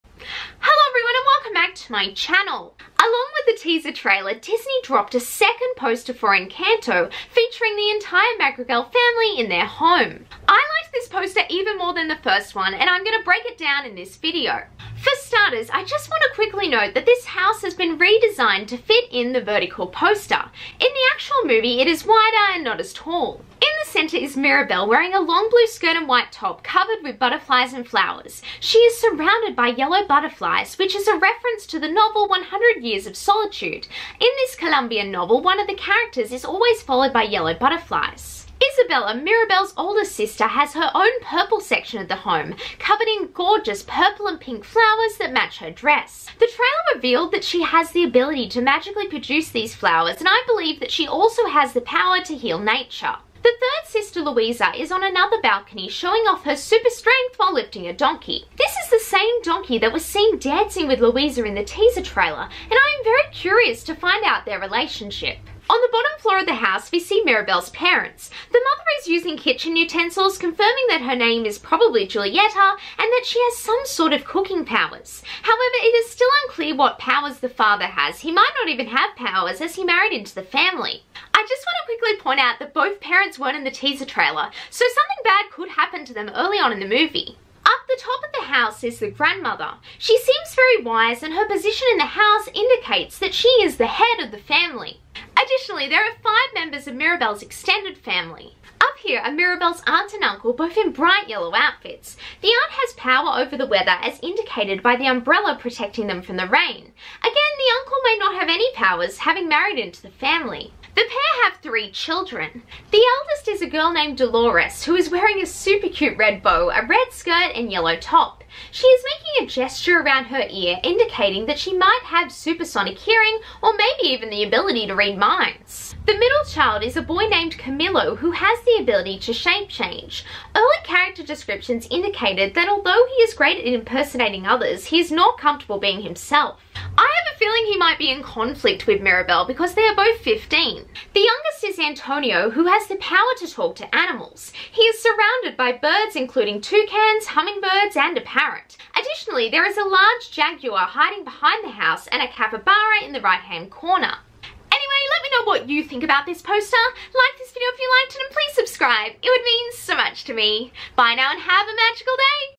Encanto | Second official poster analysis
In this video I break down the second official poster for Disney's next animated musical Encanto! It dropped at the same time as the official teaser trailer.